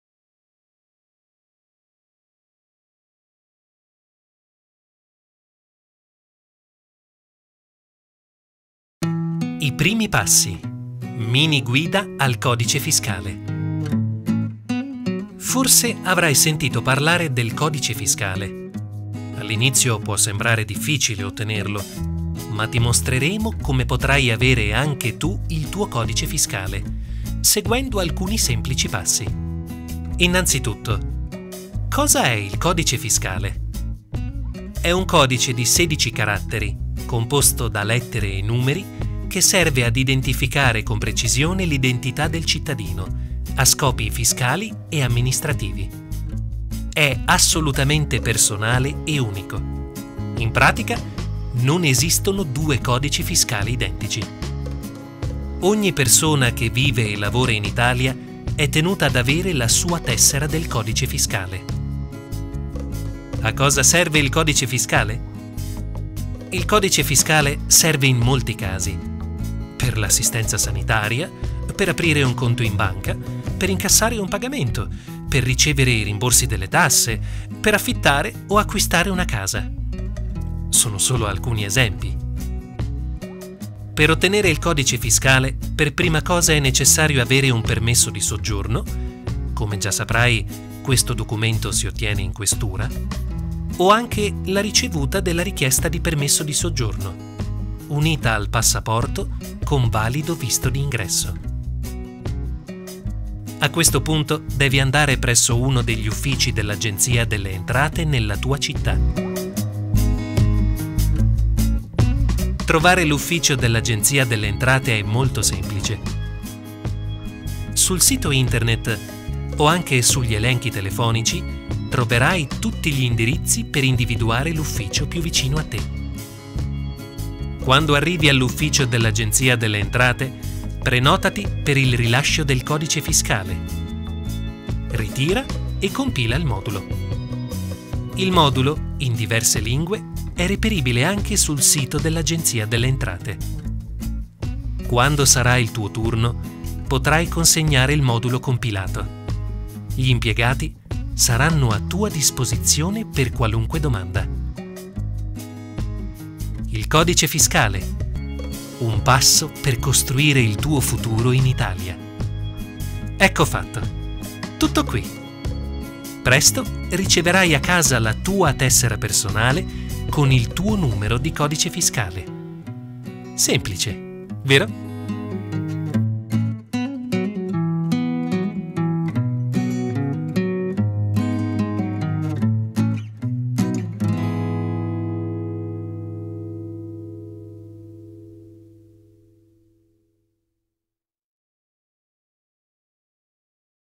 Mini-guida al codice fiscale per gli stranierivideo CF
Che cosa è il codice fiscale, a cosa serve, cosa bisogna fare per richiederlo. Sono questi i contenuti essenziali del video informativo "Mini-guida al codice fiscale", realizzato da Regione Emilia-Romagna, Agenzia delle Entrate Emilia-Romagna ed ERVET, all'interno del progetto "Parole in gioco 3- azioni del sistema regionale di sostegno alla conoscenza della lingua italiana ed educazione civica" sostenuto dal Programma FEI (Fondo Europeo per l'Integrazione) Annualità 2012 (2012/FEI/Prog-104480).
Al progetto, cofinanziato dall'Unione Europea e Ministero dell'interno, ha aderito per la prima volta la Direzione regionale delle Entrate, con l'obiettivo fondamentale di semplificare gli adempimenti per i contribuenti stranieri. La mini-guida spiega passo-passo, con linguaggio diretto, come richiedere il codice fiscale e perché è utile farlo.